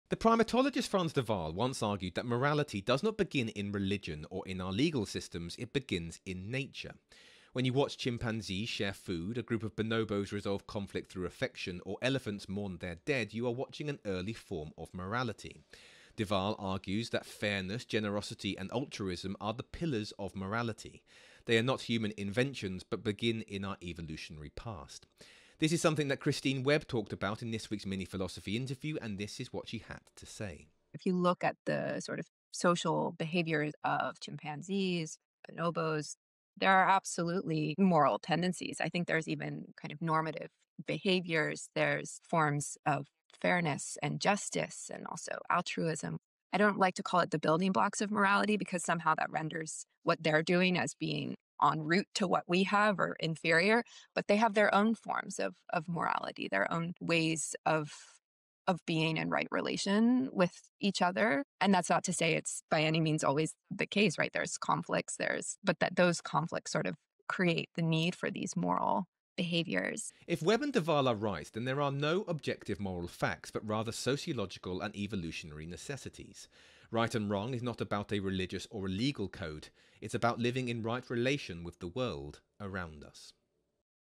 Webb and de Waal: The Roots of Morality
The primatologist Frans de Waal once argued that morality does not begin in religion or in our legal systems; it begins in nature. When you watch chimpanzees share food, a group of bonobos resolve conflict through affection, or elephants mourn their dead, you are watching an early form of morality. De Waal argues that fairness, generosity, and altruism are the pillars of morality.

They are not human inventions, but begin in our evolutionary past. This is something that Christine Webb talked about in this week's Mini Philosophy interview, and this is what she had to say: 'If you look at the sort of social behaviour of chimpanzees, bonobos, there are absolutely moral tendencies. I think there's even kind of normative behaviours. There's forms of fairness and justice, and also altruism.

'I don't like to call it the building blocks of morality, because somehow that renders what they're doing as being en route to what we have or inferior. But they have their own forms of morality, their own ways of being and right relation with each other. And that's not to say it's by any means always the case, right.

'There's conflicts, there's—but that those conflicts sort of create the need for these moral behaviours. If we want to valourise, and there are no objective moral facts, but rather sociological and evolutionary necessities, right and wrong is not about a religious or legal code; it's about living in right relation with the world around us.'